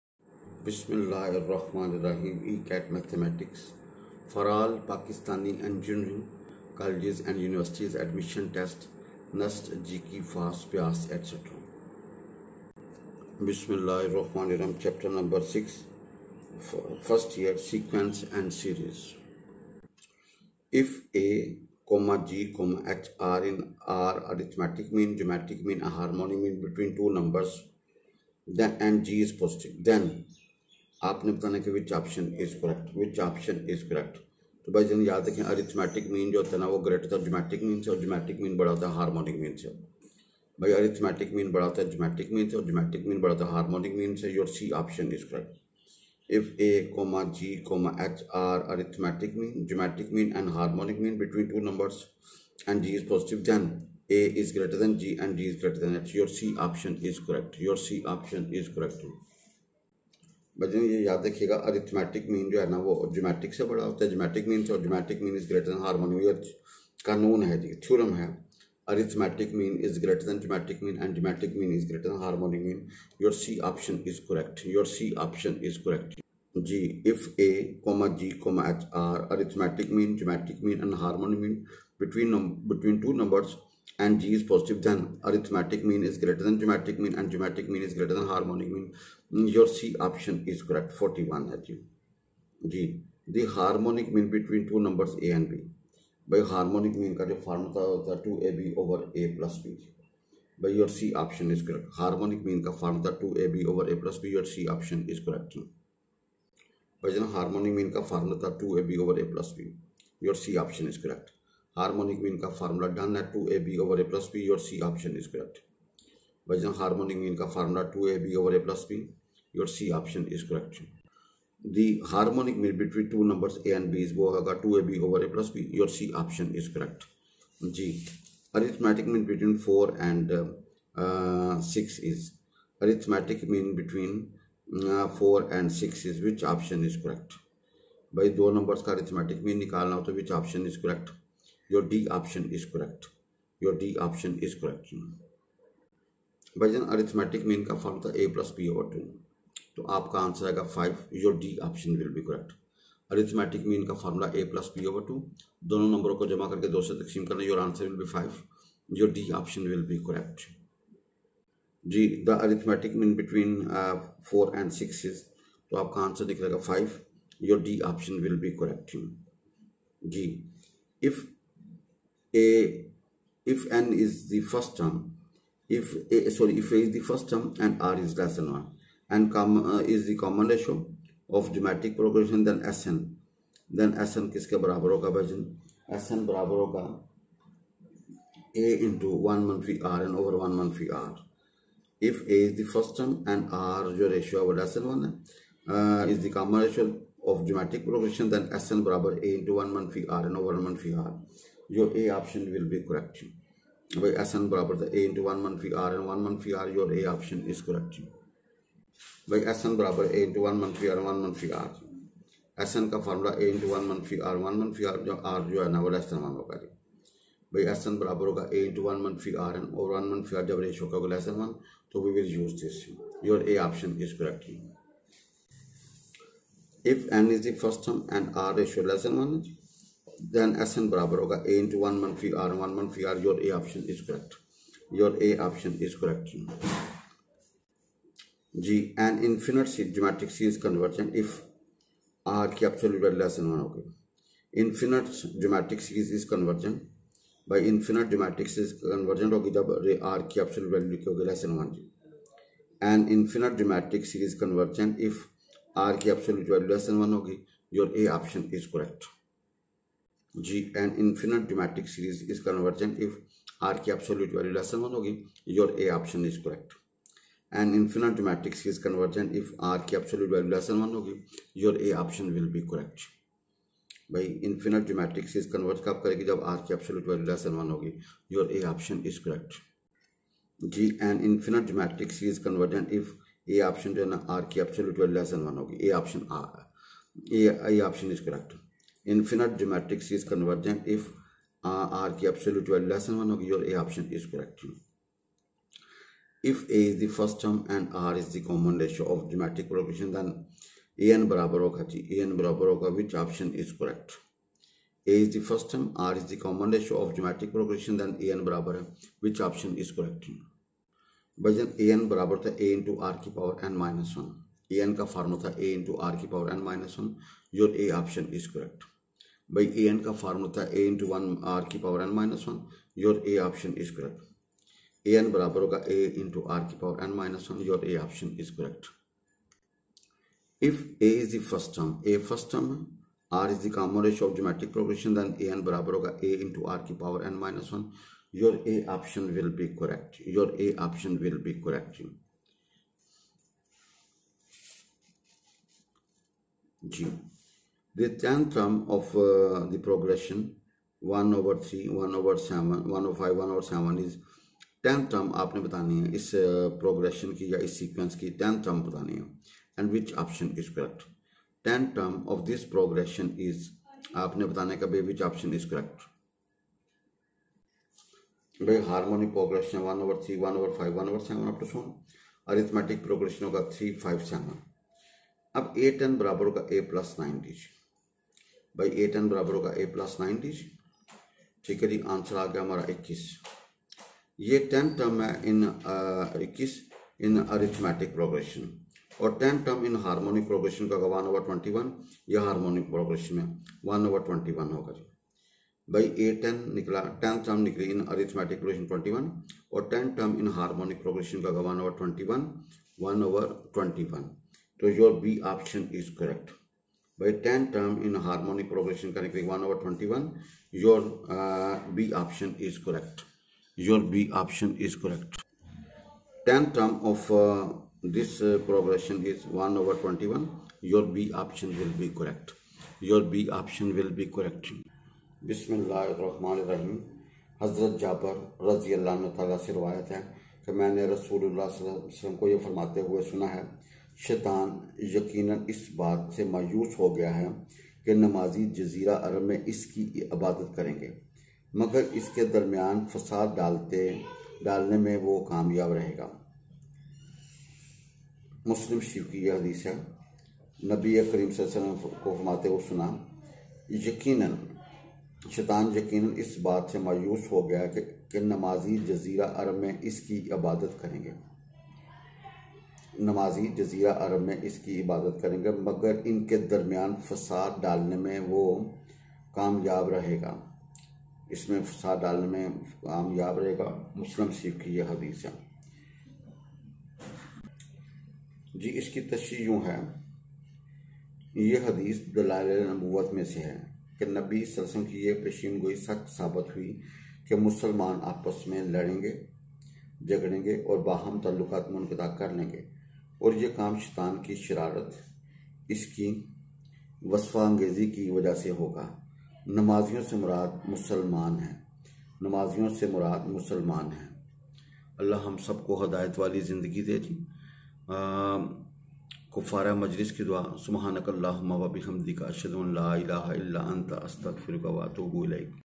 7) Maths l ECAT | F.Sc -1 | Ch # 6 Entry Test l Sequences & Series l NUST l MCQ l Fast l Giki l Lums
This video is specially made by PROF SAJID YASEEN for ECAT students ... QUESTIONs can be asked in comment section or you can also contact  on this question for any questions related maths